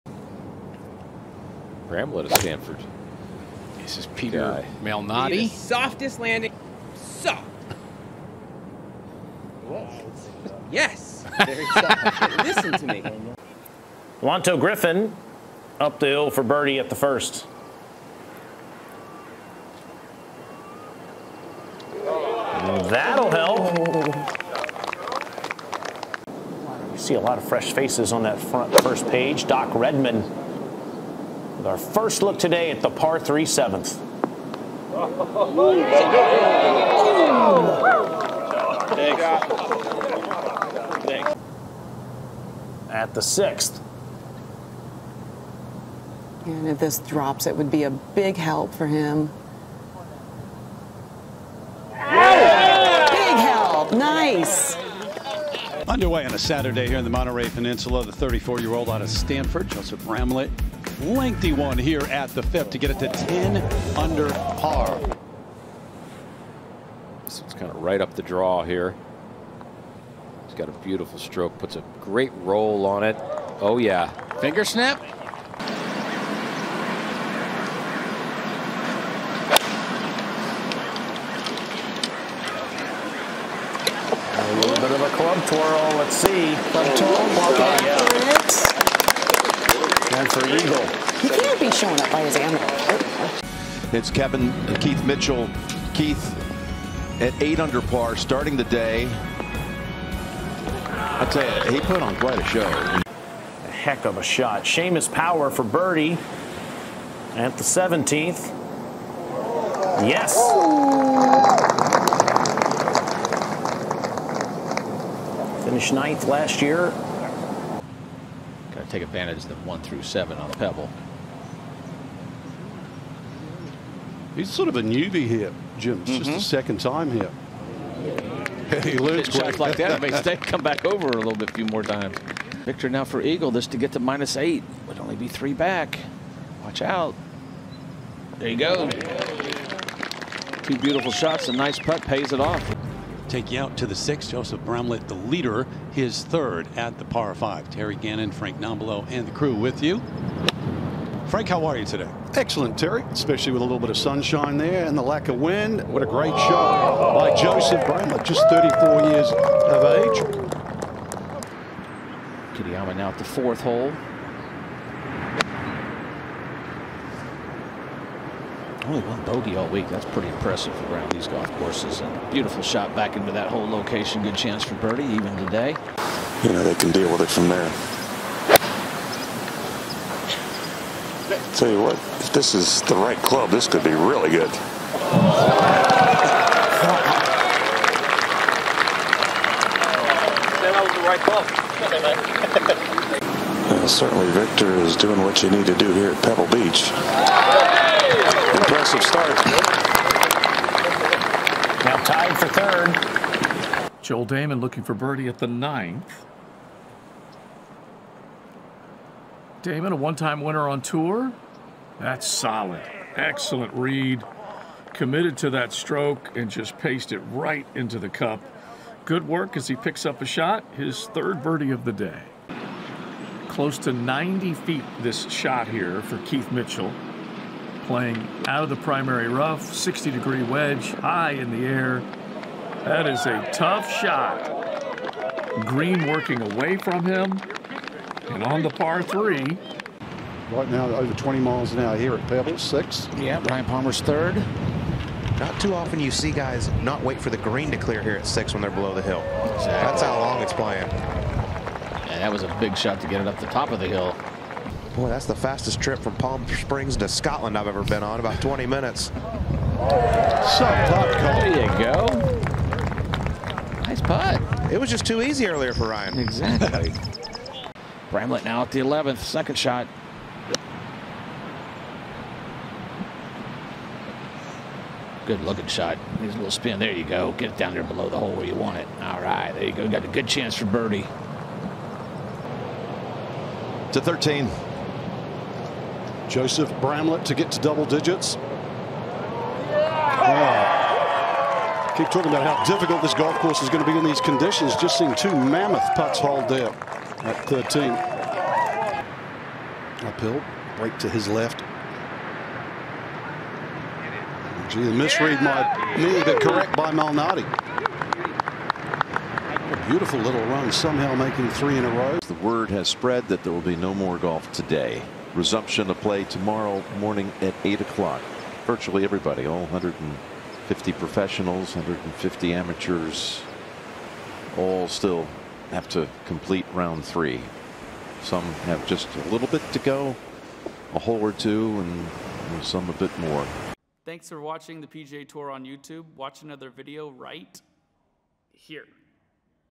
Highlights | Saturday | AT&T Pebble Beach | 2023
Check out the best shots of the day from Saturday at the 2023 AT&T Pebble Beach Pro-Am, featuring Jordan Spieth, Peter Malnati, Ryan Palmer and Viktor Hovland among others. Play was suspended due to inclement weather and will resume Sunday morning with the conclusion of Round 3.

This week, the PGA TOUR heads north up the California coast for the AT&T Pebble Beach Pro-Am. Former World No. 1 Jordan Spieth tees it up, looking to replicate his victory at the event in 2017, where he finished an impressive four shots clear of the field. Tom Hoge returns to Pebble Beach hoping to defend his lone title on TOUR. Last year Hoge shot a first-round 63 that kickstarted his tournament, leading to a two-shot victory over Jordan Spieth. The AT&T Pebble Beach will once again use its three-course rotation with the cut coming after the end of the third round. Other notable names who will be hoping to be in contention on Sunday include 2022 U.S. Open winner Matt Fitzpatrick, three-time PGA TOUR winner Viktor Hovland and Ireland’s Seamus Power, who won the Butterfield Bermuda Championship at the end of October. As is tradition, several amateur celebrities will also be taking to the course, including Pebble Beach stalwart Bill Murray, Jason Bateman, Gareth Bale and Aaron Rodgers.